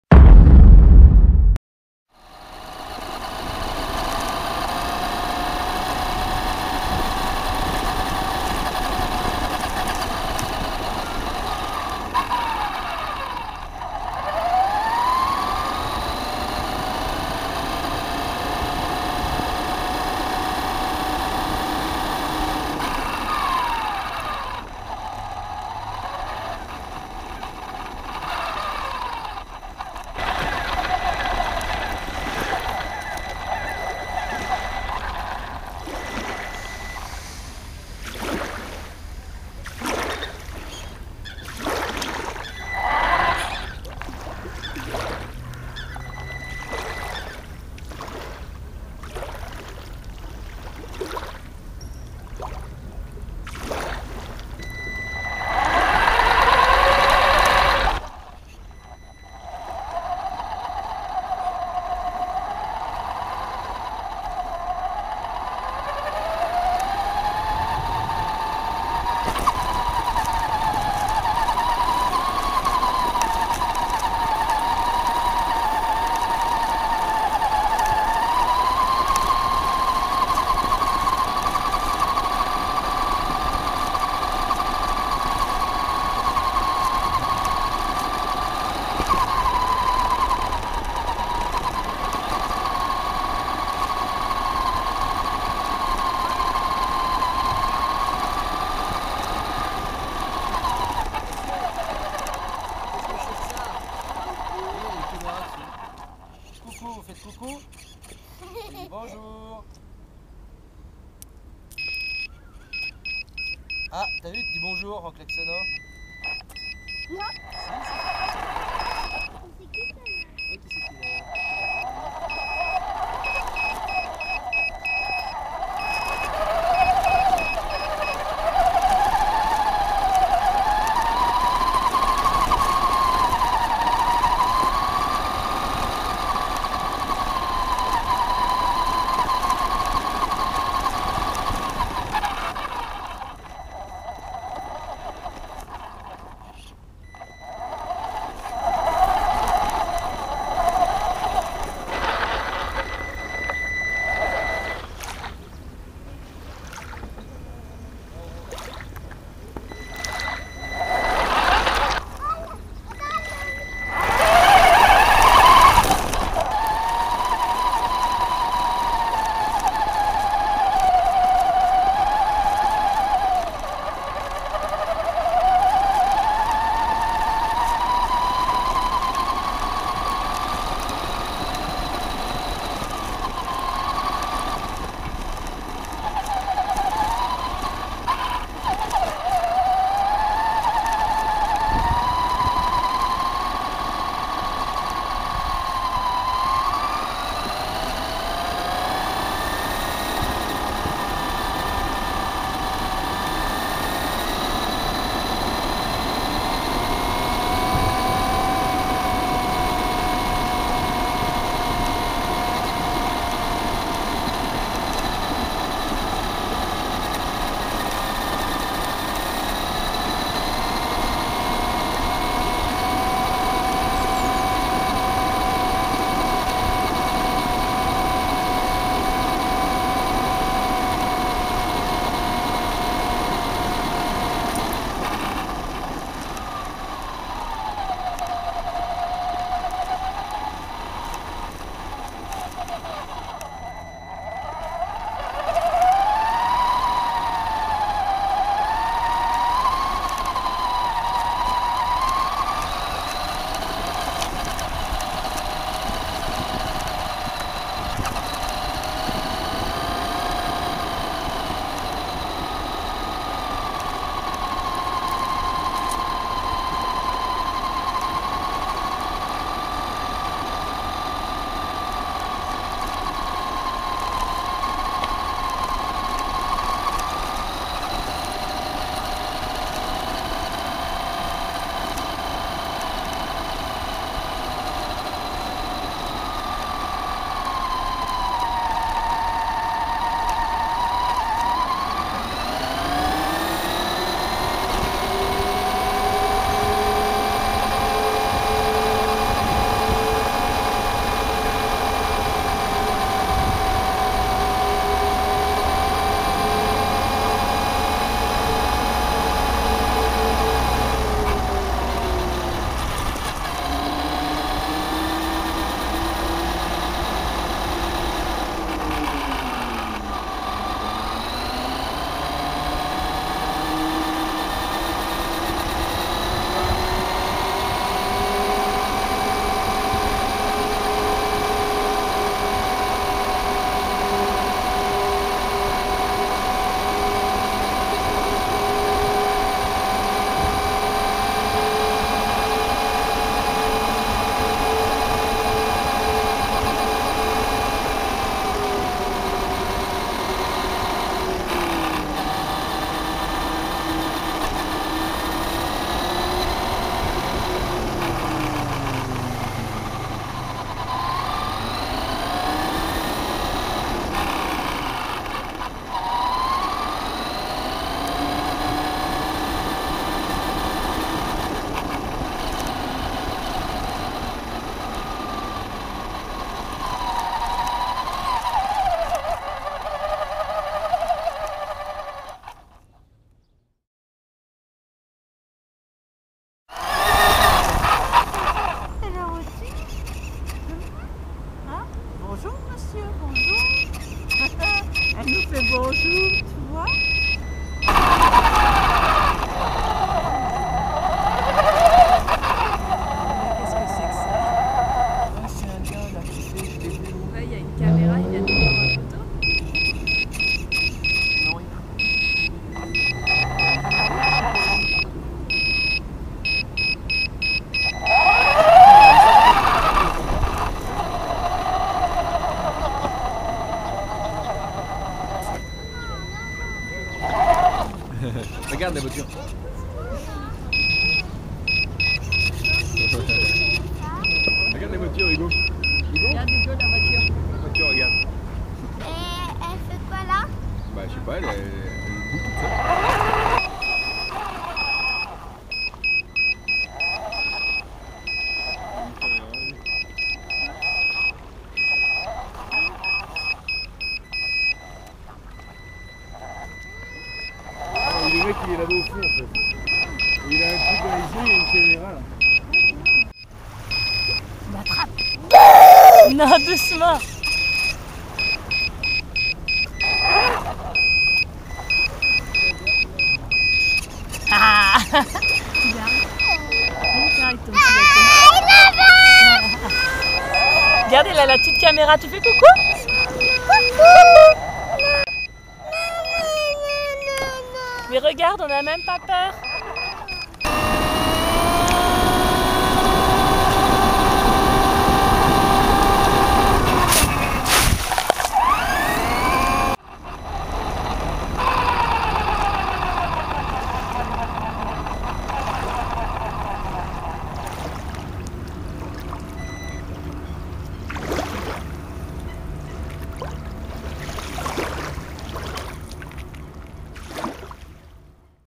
VOITURE RC A LA PLAGE ANNECY - PILOTEE EN FPV - IMMERSION - LONG RANGE
voiture radio-commandée pilotée en immersion ( FPV )
TRAXXAS STAMPEDE 4X4